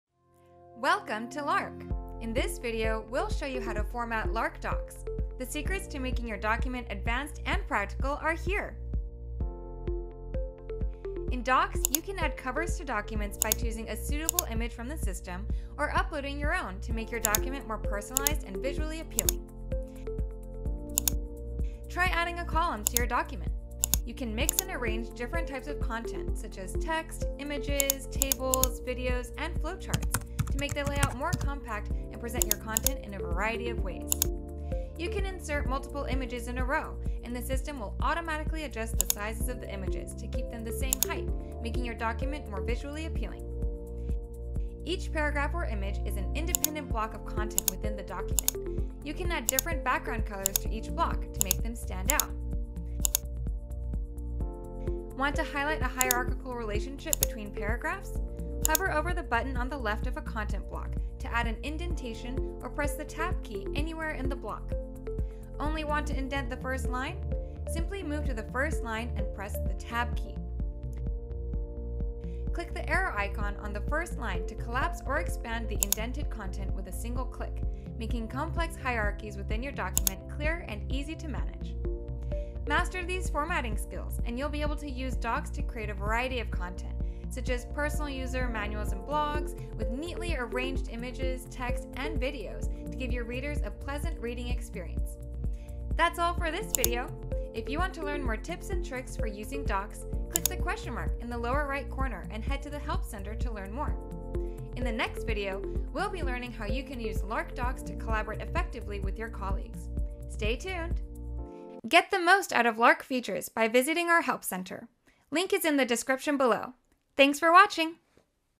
Lark 101 | Lark Docs quick guide II to advanced format | Master Document Formatting in Lark Docs
In this video, we guide you through advanced formatting techniques in Lark Docs. Learn how to personalize your documents with covers, add columns for organized content, and apply background colors to enhance readability. Additionally, we’ll show you how to use indentations and collapsible content to present your information clearly, making your documents more professional and easy to manage.
Try Lark for free and find the perfect plan for you

00:00
00:13 add cover to document
00:22 add a column
00:38 automatically adjust the sizes of the images
00:46 add different background colors to each block
00:56 add an indentation
01:06 press the tab key